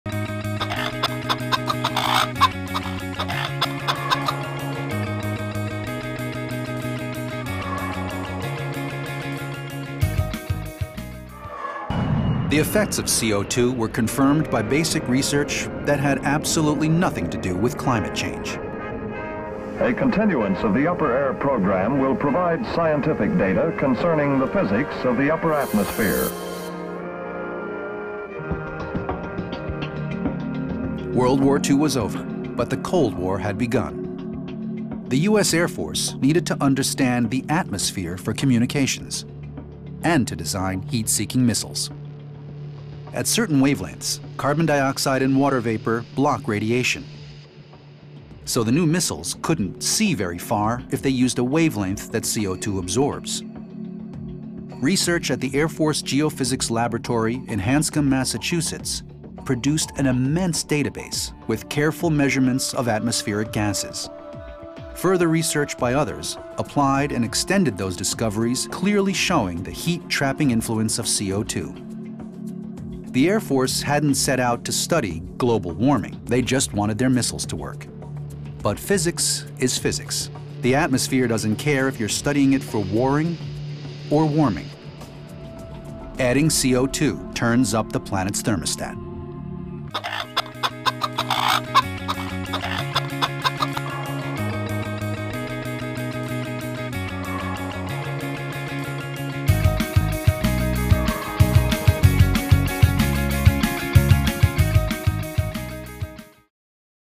How to Talk to an Ostrich: "Who says CO2 heats things up?"
Some seem to think that the heat-trapping properties of carbon dioxide are exaggerated or are just a scientific phenomenon. But after World War 2 it was the US Air Force that studied CO2 most carefully: it's heat-trapping properties could interfere with heat-seeking missiles. As the video says, "the atmosphere doesn't care whether you study it for warring, or warming. Adding CO2 turns up the planet's thermostat."